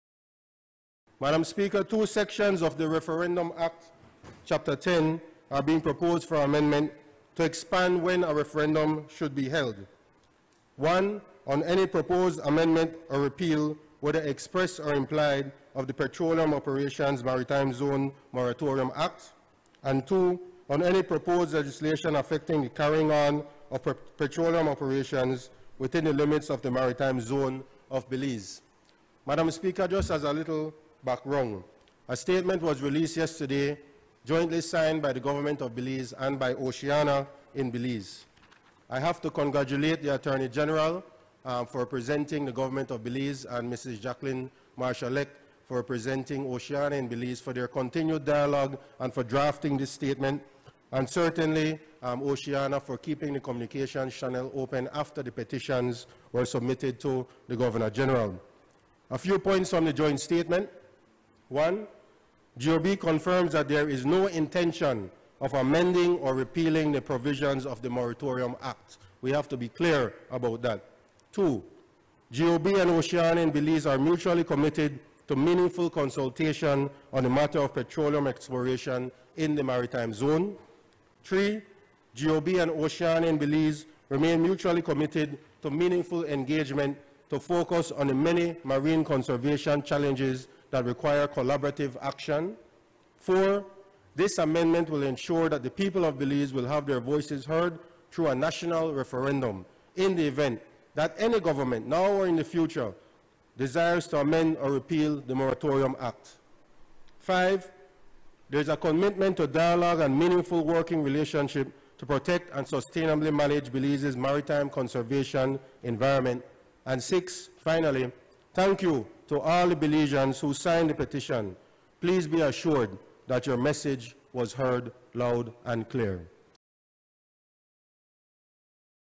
Government Introduces Bill to Amend the Referendum Act Pertaining to Offshore Drilling
On Thursday, we reported on the joint statement by the government and OCEANA in which both sides stated that they will respect the demand of the people for a referendum on offshore oil drilling. The government had promised that if it ever changed its position on offshore oil exploration, it would go to the public in a referendum first. And in the joint statement, it also said that it would introduce a bill in the House today to amend the Referendum Act so that it makes a referendum mandatory prior to any proposed amendment or repeal of the Petroleum Operations Maritime Zone Moratorium Act. Minister of Constitutional and Political Reform, Henry Charles Usher introduced the bill for that amendment in the morning session of today’s House Sitting, after which the bill was taken through all its stages forthwith, on a unanimous vote by parliament.